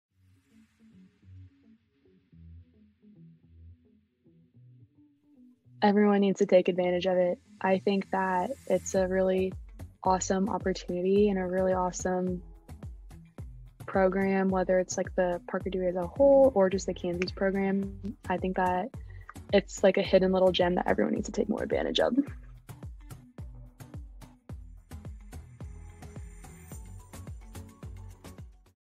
Lily's Final Thoughts on Parker Dewey
Meet Lily, a rising junior at Kansas State University selected for seven Micro-Internships. We chatted with Lily about the Kansas Micro-Internship Program, how her academic coursework has connected with her hands-on project experiences, and her advice for students and Kansas companies considering Micro-Internships.